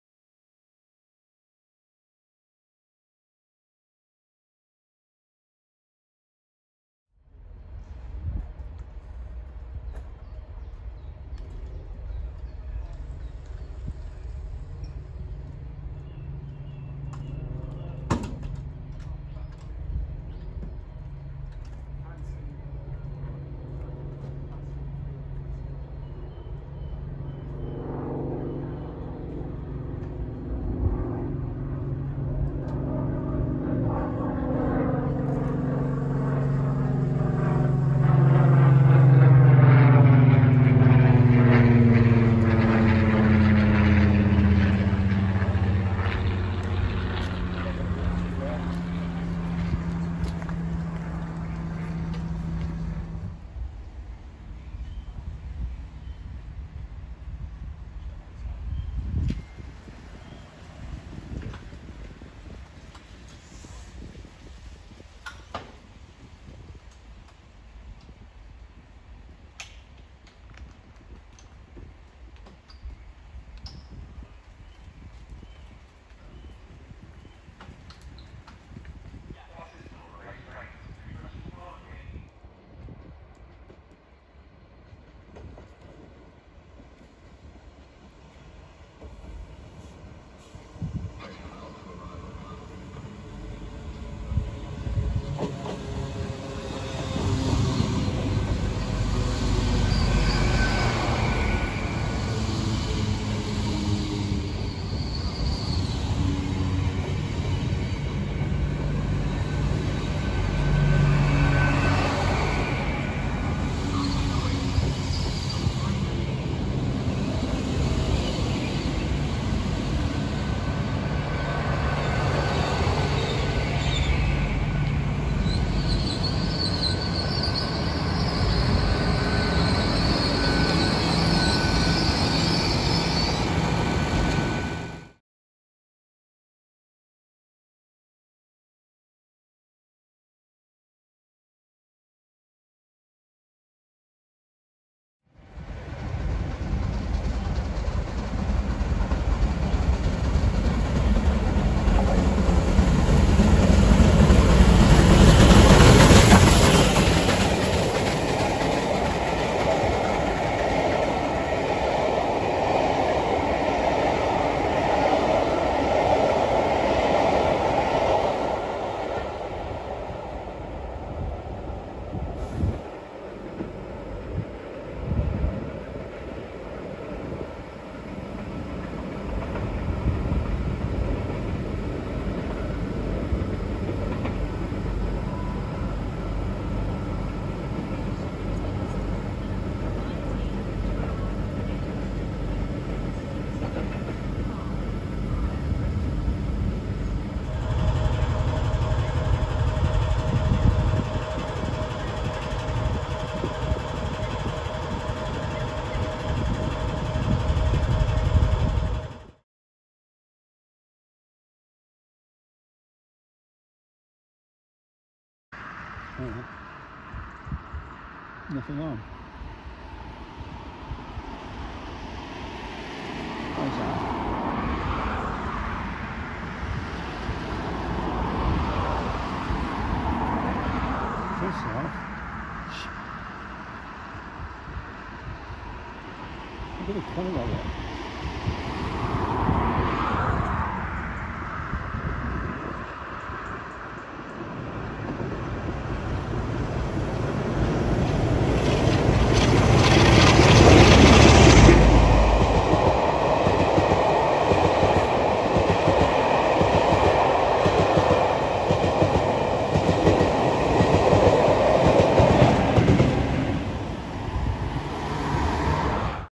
Westbury: Clan Line 'Atlantic Coast Express' 2/7/22
Clan Line passing Westbury and other activities in the area including the 'English Riveria Express' at Frome.

Highlights:

- Colas Class 70s close up.
- The sound of a Spitfire!
- 'Clan Line' passing through to Warminster and Waterloo.
- Top n' tail class 47s in retro liveries.